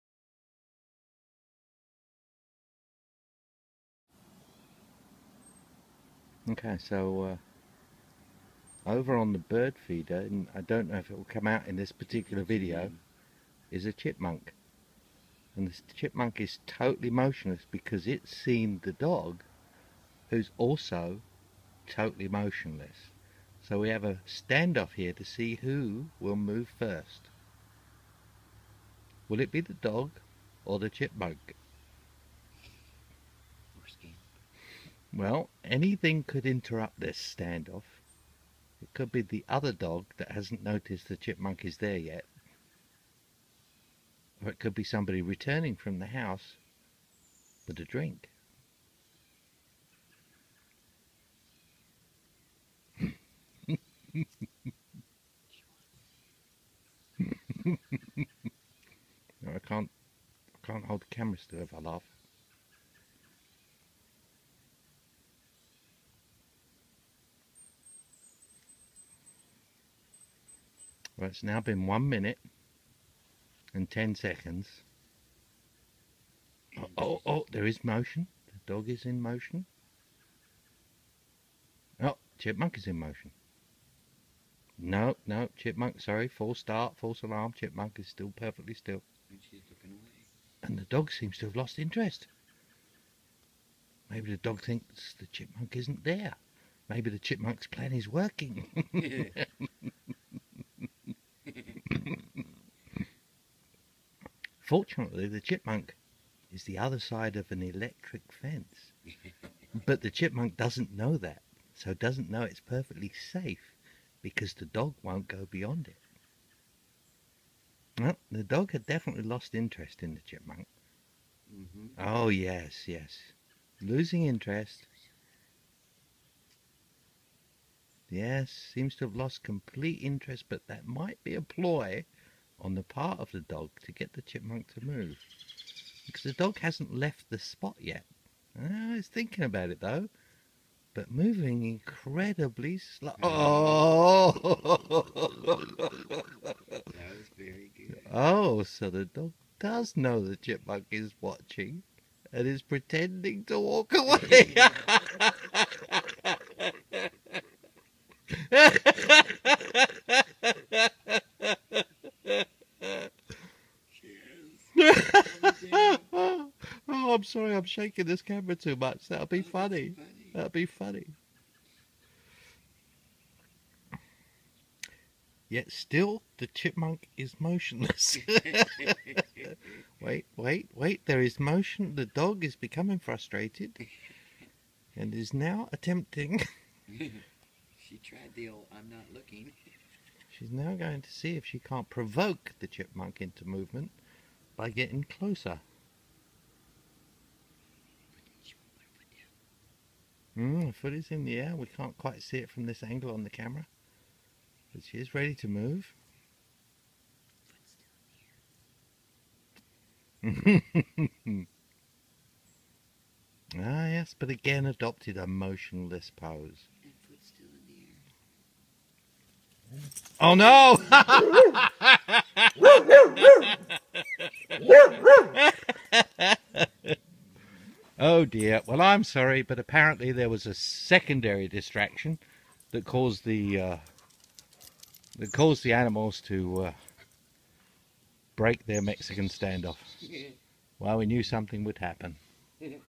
Mexican Standoff - Dog vs Chipmunk
Our English Setter doing an excellent job of trying to get a response from a very still Chipmunk.